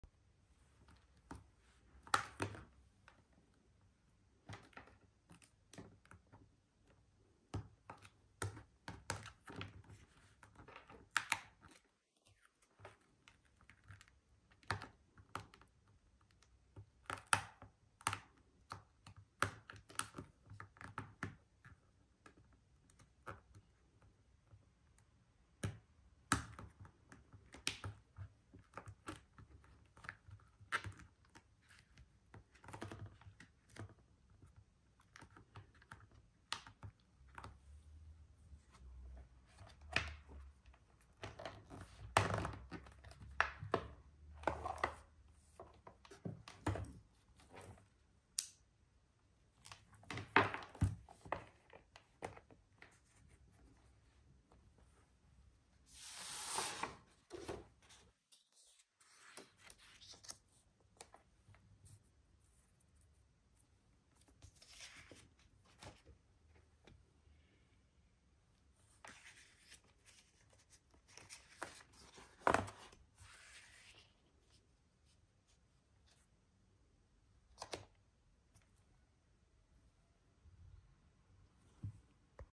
DIY LED Drawing board
I made a LED drawing board using A4 plastic file box and a 3 meter LED strip. Now it’s easy and fun to recreate/reproduce drawings.